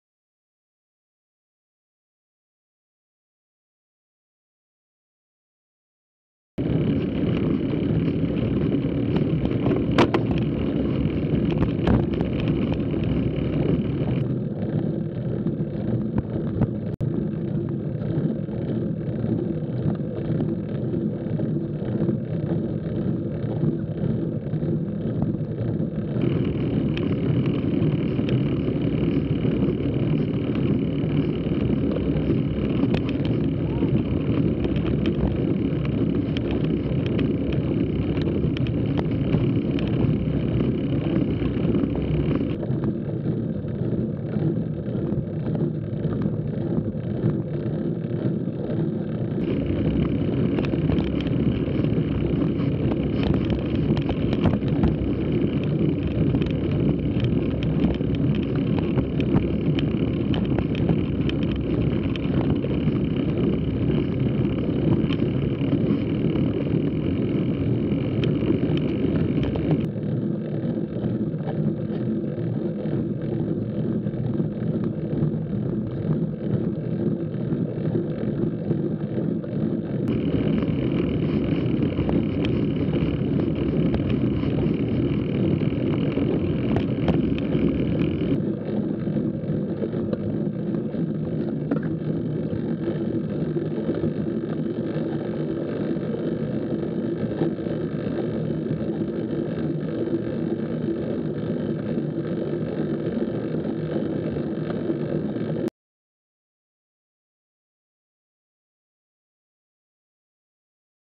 Cannondale-Garmin Flèche Wallonne Recon: Mur de Huy!
The Cannondale-Garmin squad is ready for Fleche Wallonne following the recon ride. All footage filmed on the Garmin VIRB. Front facing camera and data from Matej Mohoric, Back facing camera and data belong to Alex Howes.